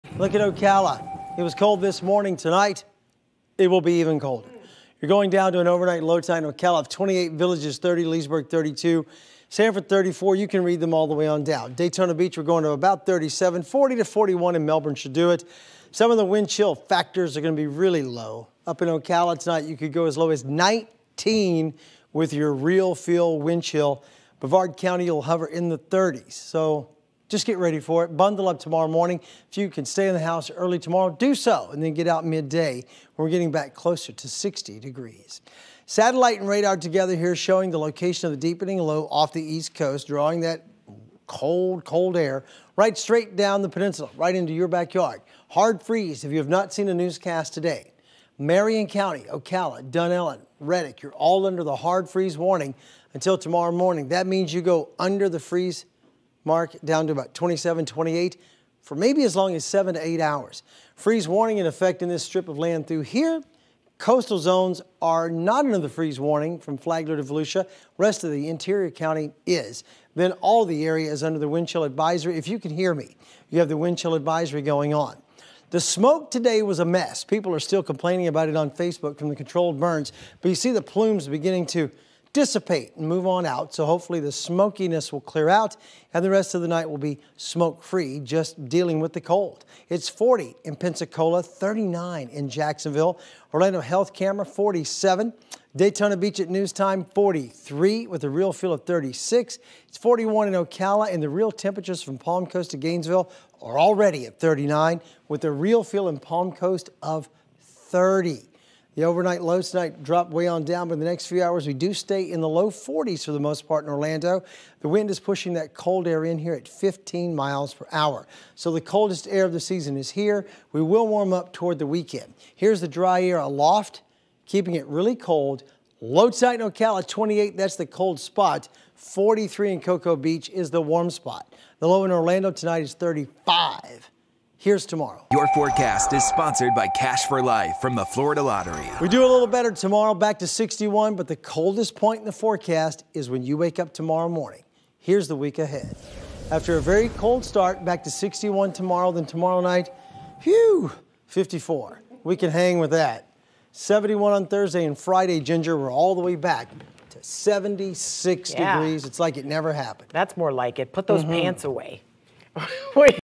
Tuesday marks coldest night of Winter in Central Florida so far
Temps are dropping throughout Central Florida.